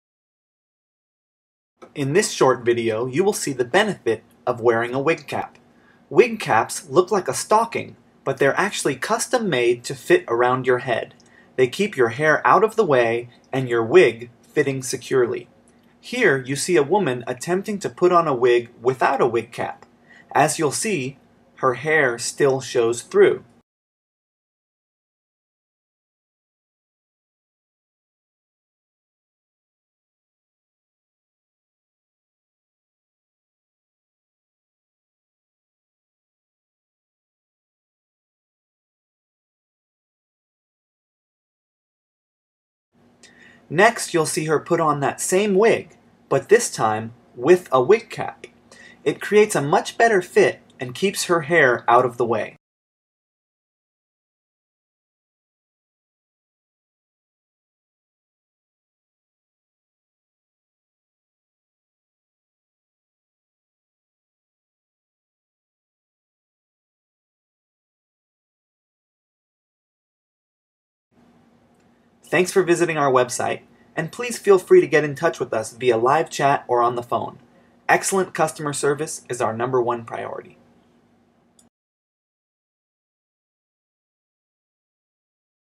Wig Cap 101: The Benefits Of Wearing A Wig Cap
offers the largest selection of costume wigs anywhere on the internet. In this short video you will learn the benefits of wearing a wig cap.